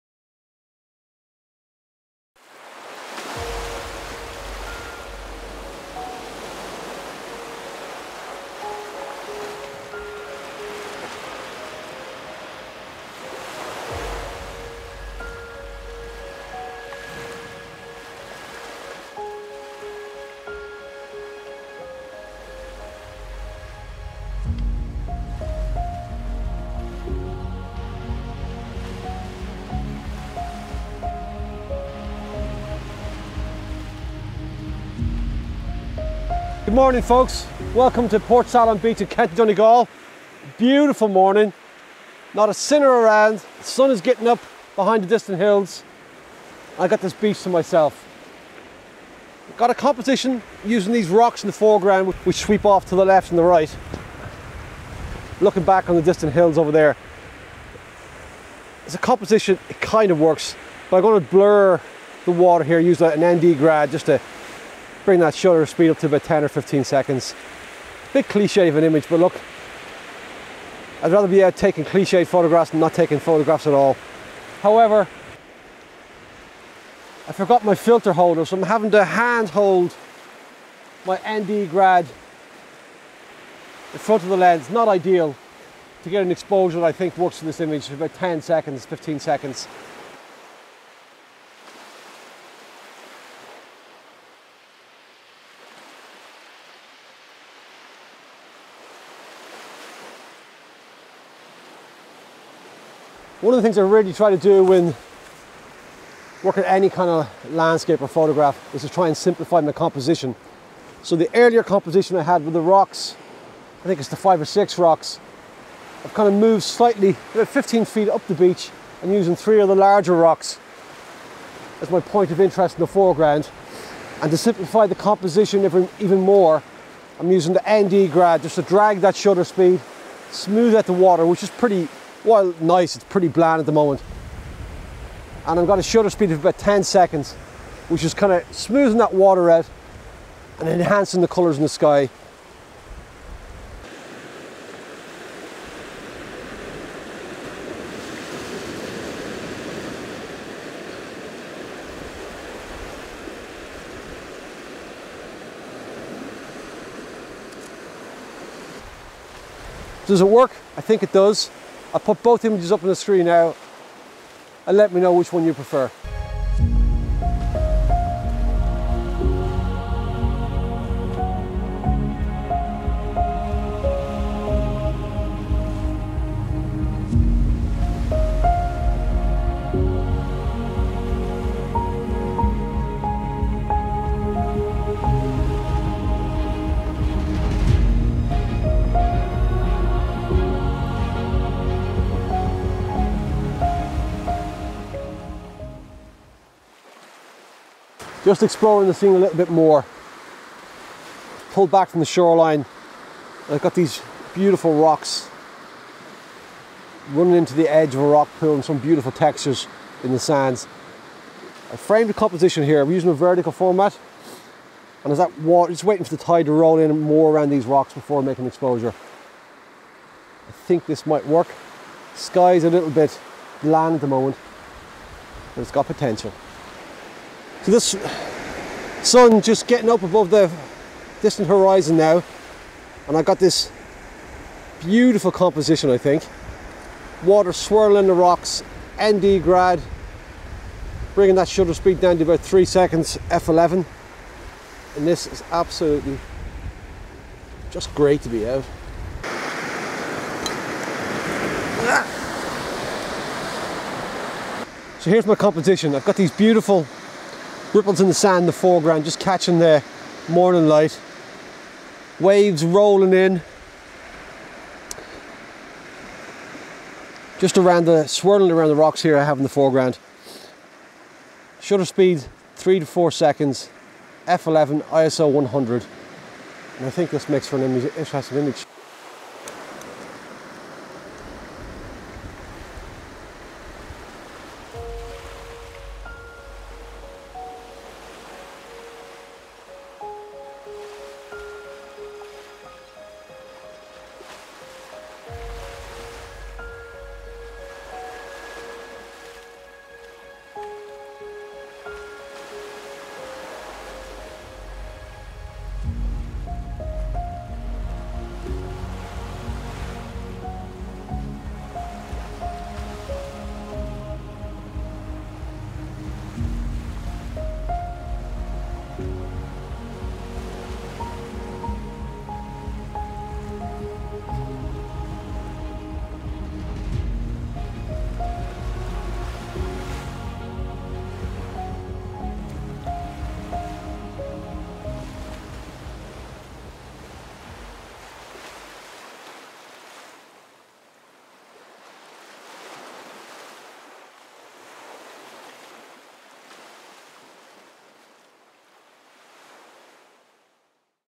Best Beach in Ireland For Sunrise ? | Landscape Photography
Landscape Photography on Portsalon Beach in County Donegal Ireland. With a camera in hand, and with views overlooking Lough Swilly it is perhaps one of the best beaches in Ireland to start the day.